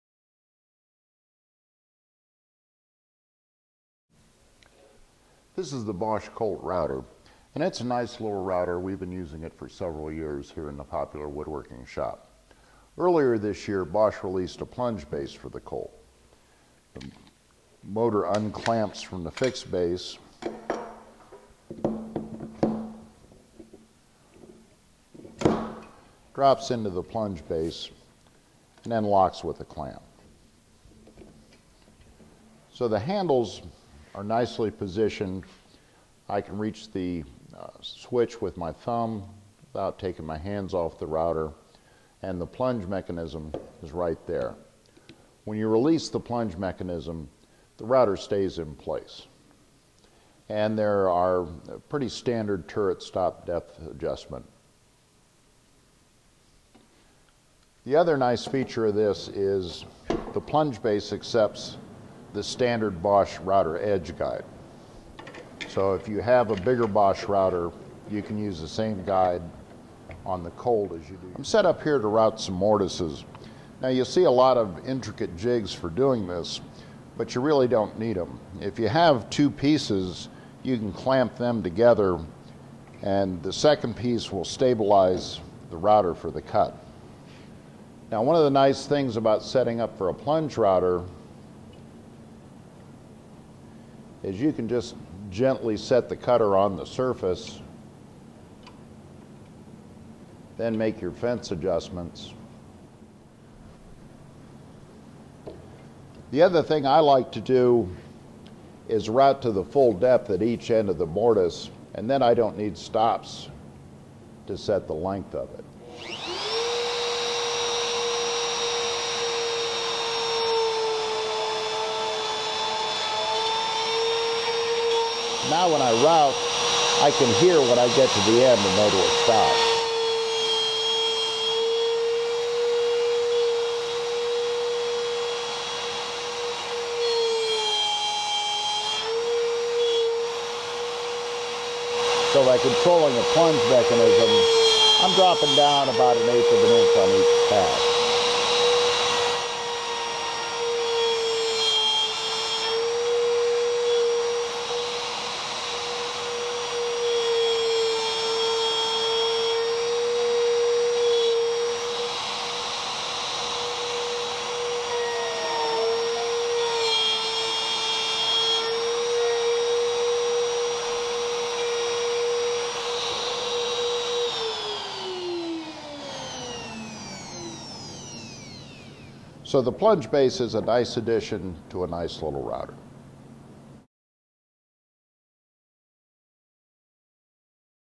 Bosch Colt Router Plunge Base
Robert W. Lang takes the Bosch Colt Plunge base for a test drive making mortises.